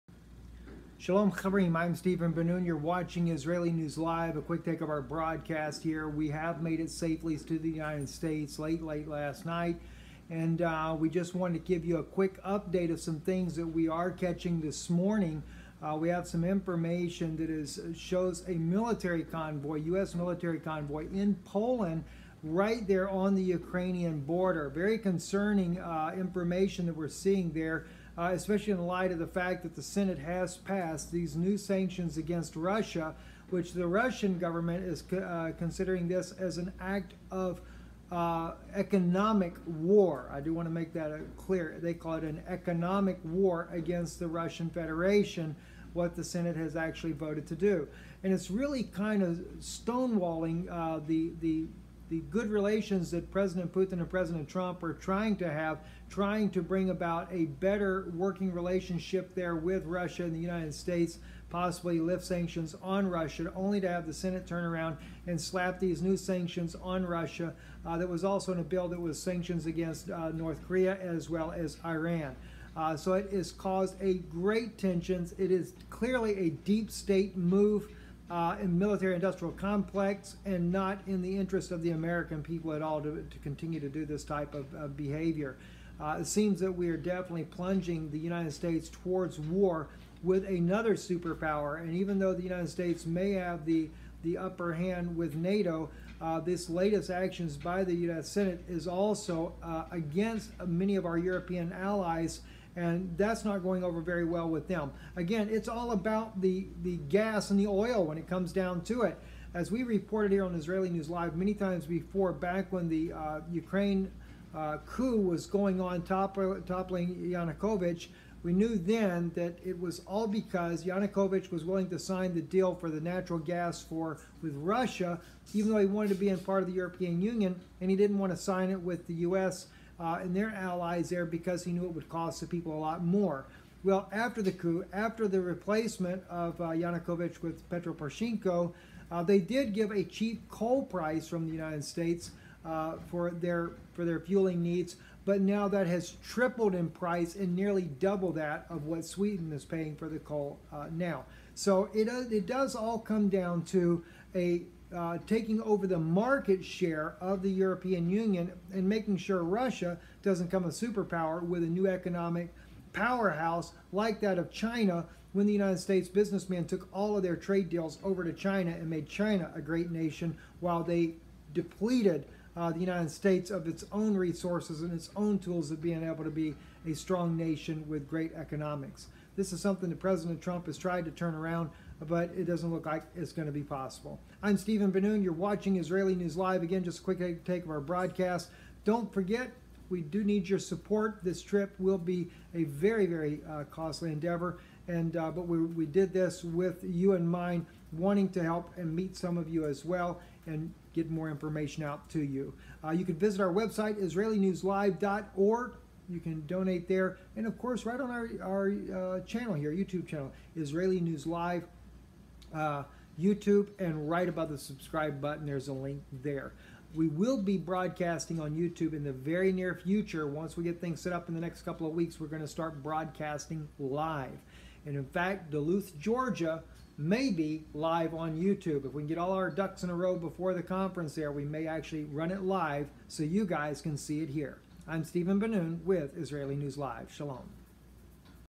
US Military Convoy on Ukraine Border
Do US military industrial complex certainly doing nothing to help President Trump and trying to restore relations with President Vladimir Putin of Russia. A military convoy sing in Poland on the border of Ukraine after the recent sanctions passed by the Senate makes one wonder what will come next.